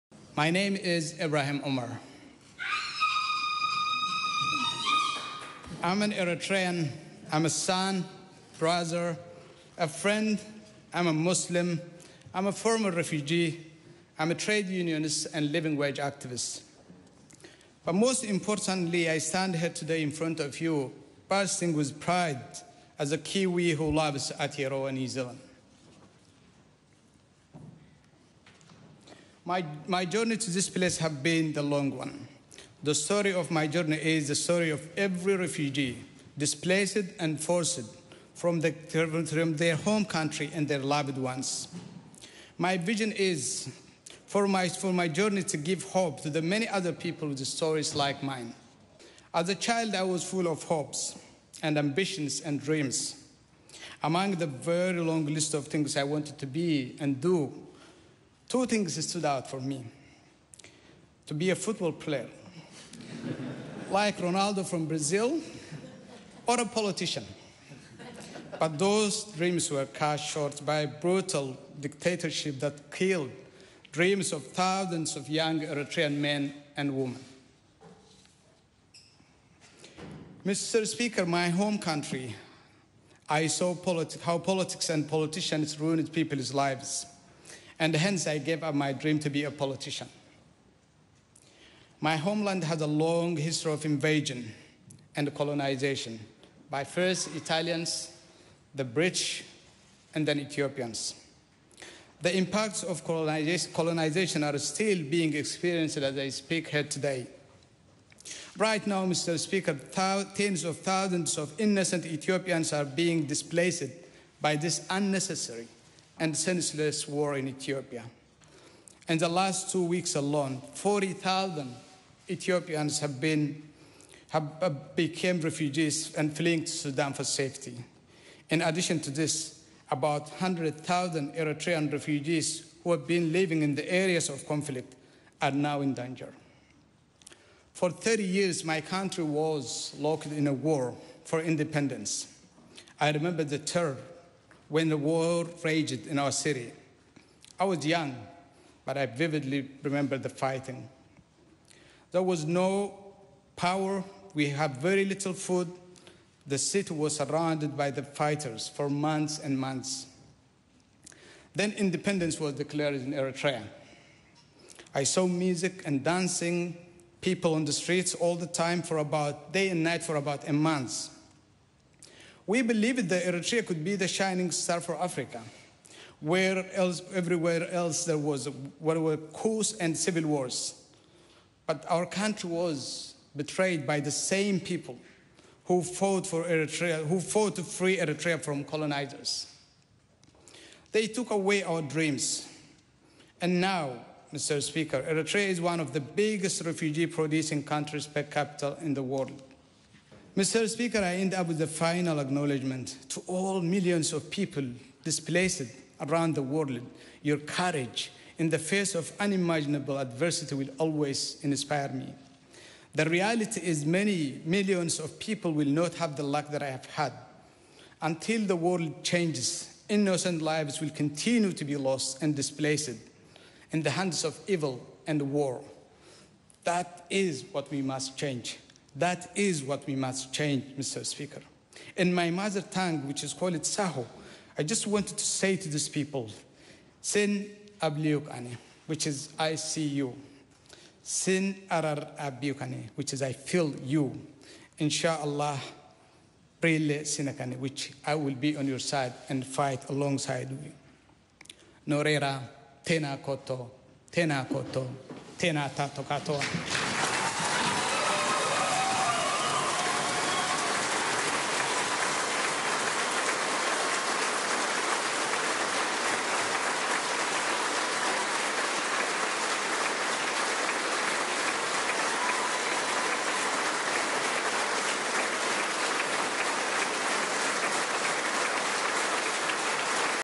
MP Ibrahim Omer Delivers First Speech at New Zealand Parliament | NowThis
‘I’m an Eritrean, a son, a brother, a Muslim, a former refugee ... the story of my journey is the story of every refugee.’ — Watch MP Ibrahim Omer’s moving first speech at the New Zealand Parliament. Omer is the first African MP and second refugee MP in NZ history.